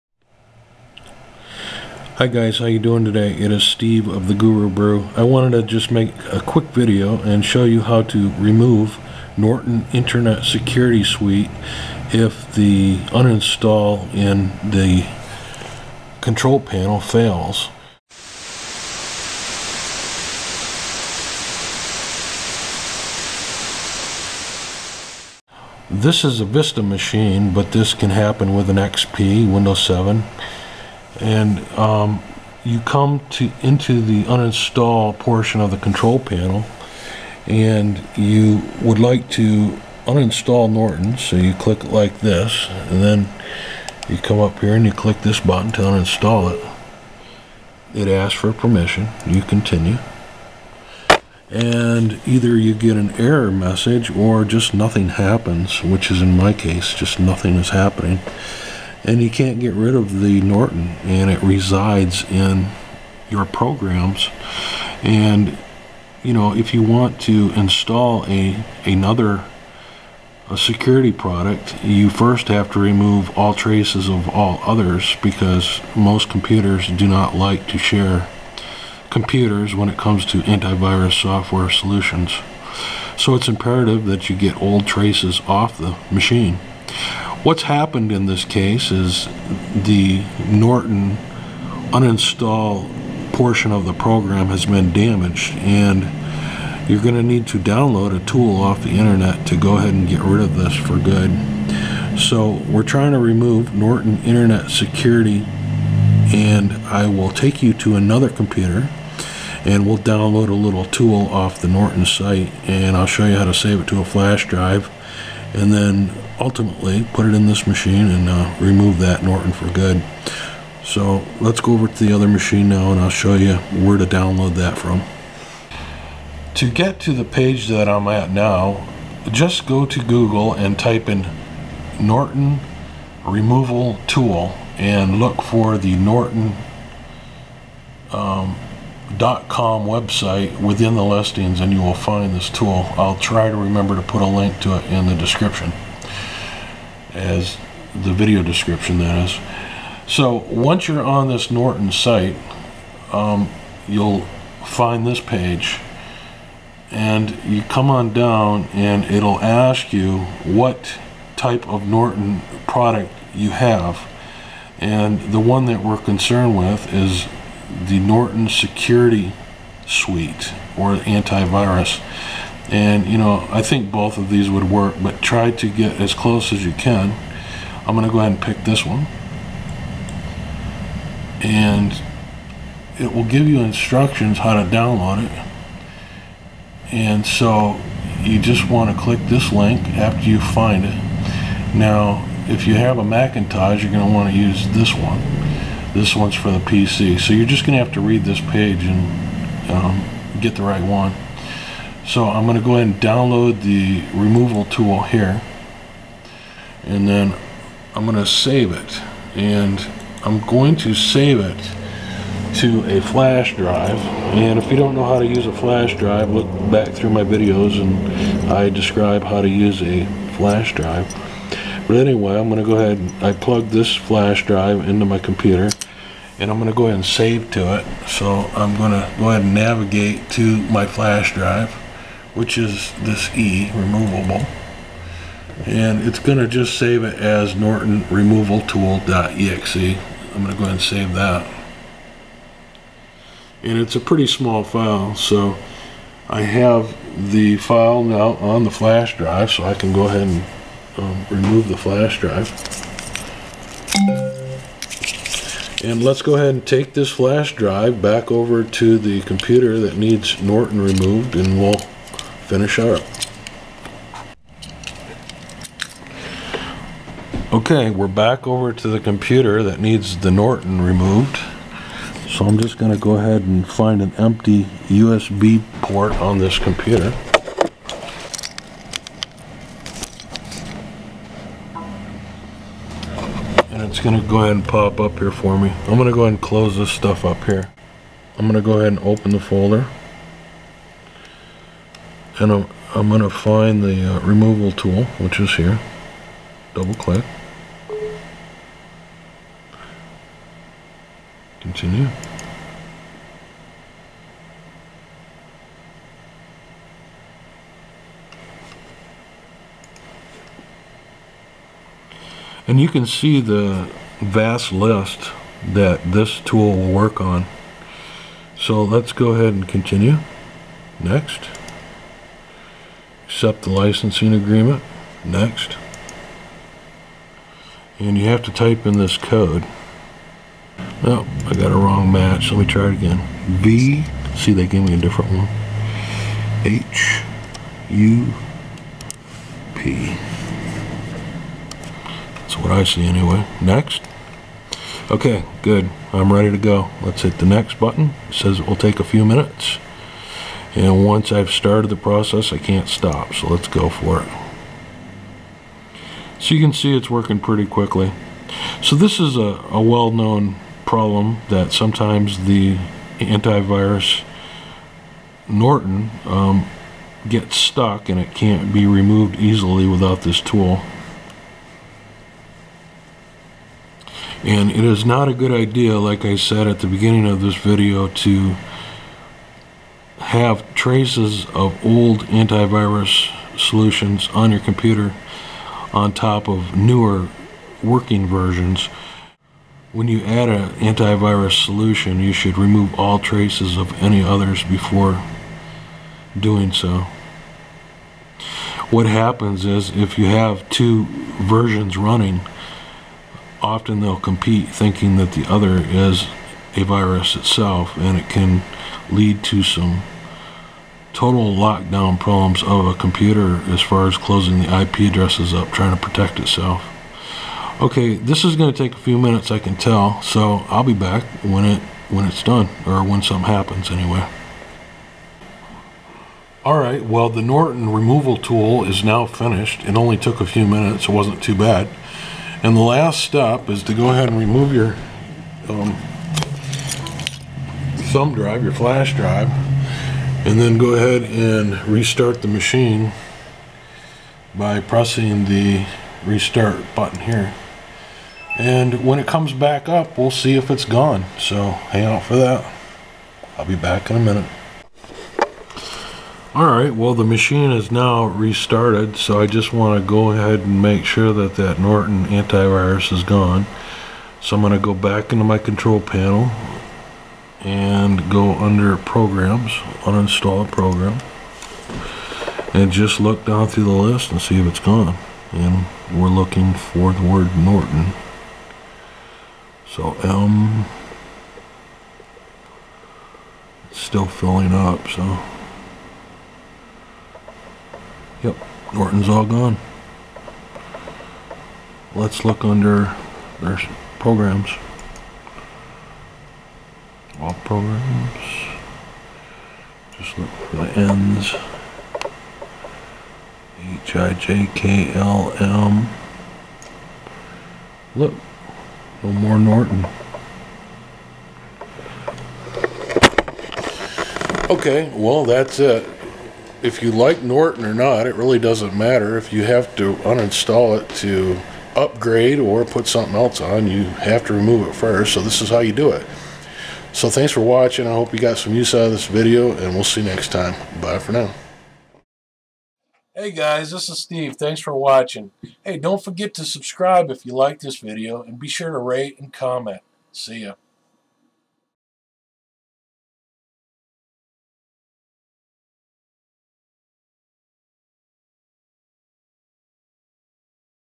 How to Remove Norton Security Products if Uninstall Fails
This video describes how to remove Norton security software from you computer if uninstalling it traditionally using the add/remove programs has failed. The Norton Removal Tool uninstalls Norton Anti Virus, Norton Internet Security, Norton 360, and Norton System Works 12.0 completely from your computer.

Download Norton Removal Tool to remove a failed installation or a damaged Norton product that cannot be removed by traditional means.

Link to removal software from Norton that has links for a PC or Mac